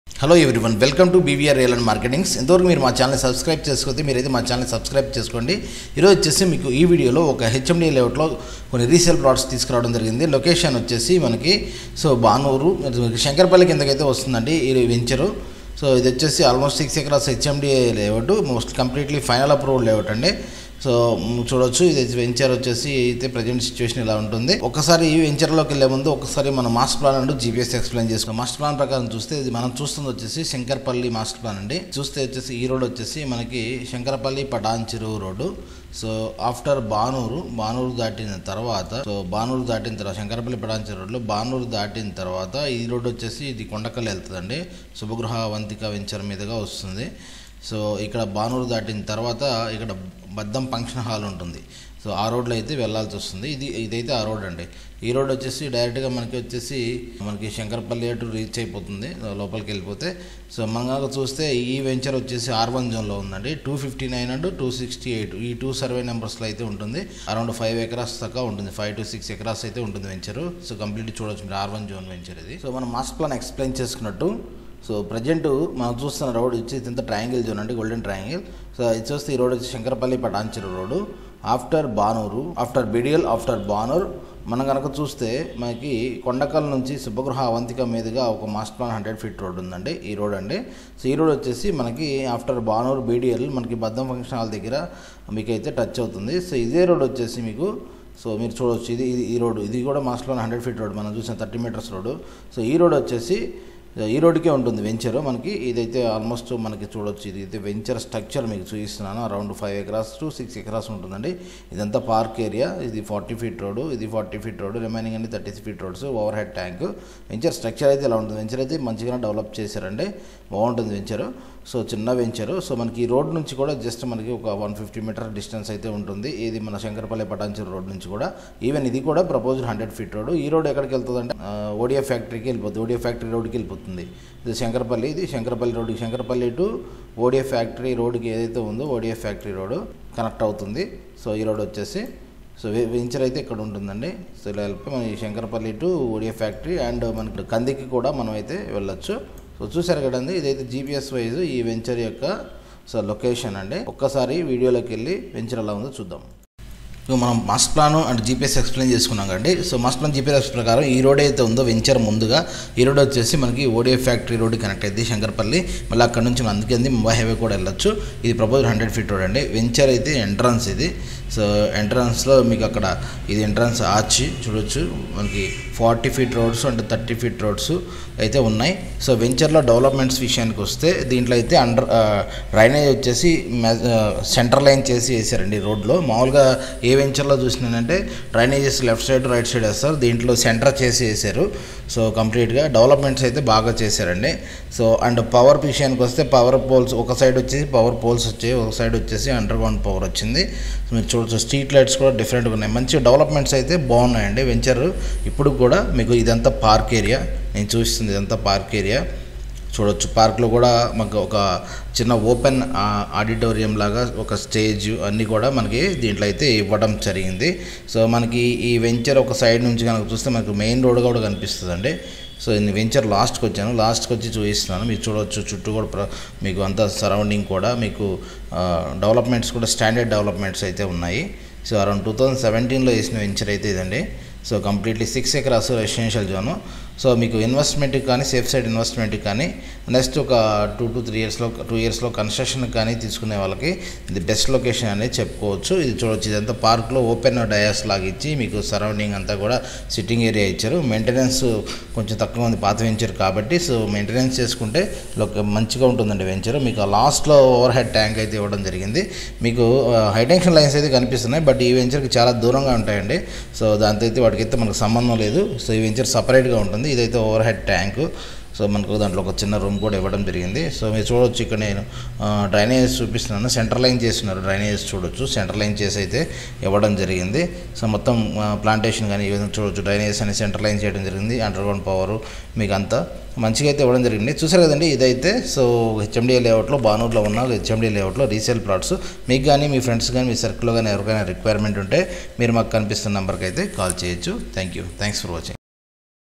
AVALANCHES INFRA hmda ReSale Plots | Shankarpally | 100 FT Road | R1 Zone
GO TO OUR Website link for location layout video
HMDA LP NO: 000036/L0/PLG/HMDA/2017

Total Area Of Venture : Around 5 Acr's HMDA Final Approved

Below are Available Plots Details :

Plot Size       :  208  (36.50*50) Sq.Yrds EAST
Road Size     : 30 Feet
Price Range  : 33,000/- Per Sq.Yrd Negatable .

Venture Developments :
Semi Gated Venture .
Under Ground Power
Street Lights .
100/40/,30 Feet Rods
Parks: OTH Tank
Trees Plantation with good Environment .
Land Provision for Swimming pool and Club House
Social Infra Play Area
300 Metres To Shankarpaly To Patancheruv Master Plan 100 FT Road
11 KM To Ptancheruv Bus Stand & Mumbai highway
5 KM TO RR7
6 KM TO MOKILA
Near To Lahari Resorts
2 KM To Bhanur
28 KM to HiTech City Via Patancheruvu
2 KM To Shankarpally

Related Properties You Can Search Here From Below Given links:

Other Ventures in hyderabad for Location and Layouts as well price List go here

Thanks & Regards,
BVR Real & Marketing's